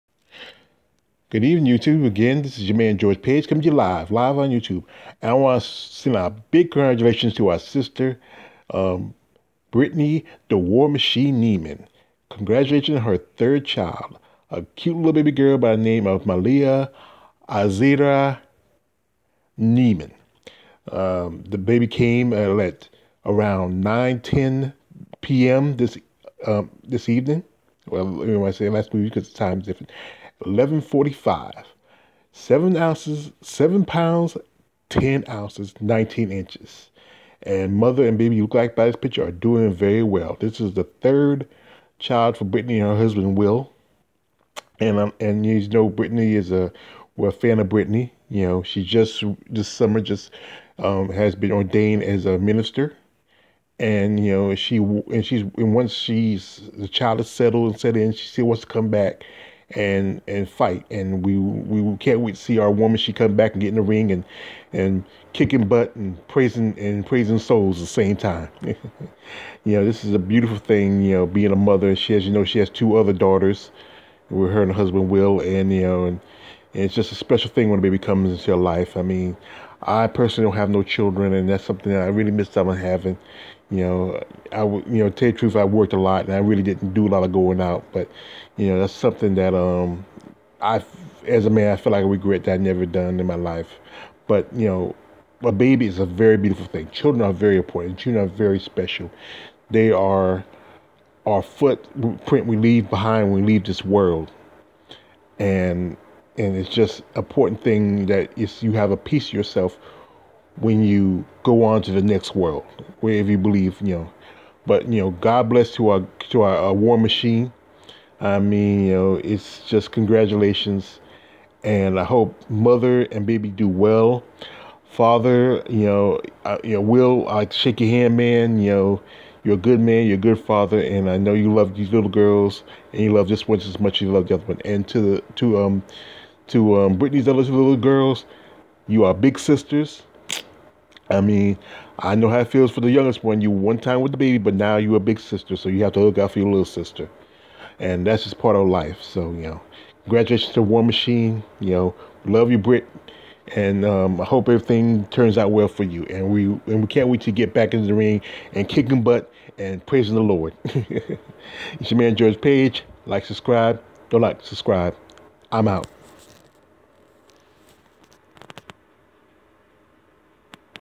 Congrats to the Warmachine on her New Baby 😄🍼🎉🎊🎀
Maleah Azaria Nieman
Date: 11/4
Time: 910pm
Weight: 7.10lbs
Lenght: 19inches

God Bless you and your new baby.